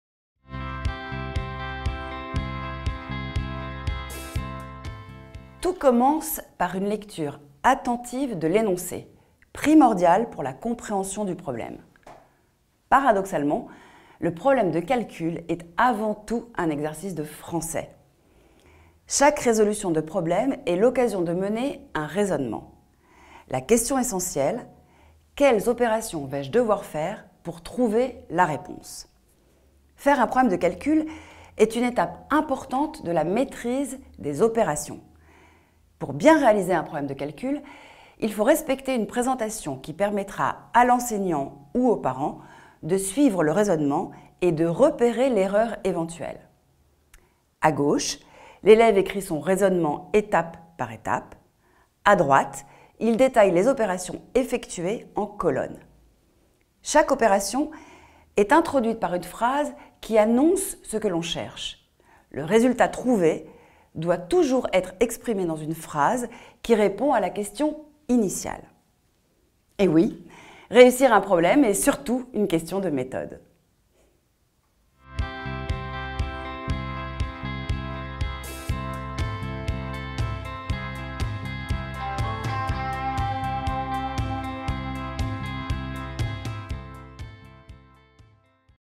Comment aider mon enfant à résoudre un problème de mathématiques
Tout commence par une lecture attentive de l’énoncé, primordiale pour la compréhension du problème. Paradoxalement, le problème de calcul est avant tout un exercice de Français.

Chaque résolution de problème est l’occasion de mener un raisonnement. La question essentielle : « Quelles opérations vais-je devoir faire pour trouver la réponse ? ».

Faire un problème de calcul est une étape importante de la maîtrise des opérations.

Pour bien réaliser un problème de calcul, il faut respecter une présentation qui permettra à l’enseignant ou au parent de suivre le raisonnement et de repérer l’erreur éventuelle. A gauche, l’élève écrit son raisonnement étape par étape. A droite, il détaille les opérations effectuées en colonnes.

Chaque opération est introduite par une phrase qui annonce ce que l’on cherche. Le résultat trouvé doit toujours être exprimé dans une phrase qui répond à la question initiale.

Réussir un problème est surtout une question de méthode !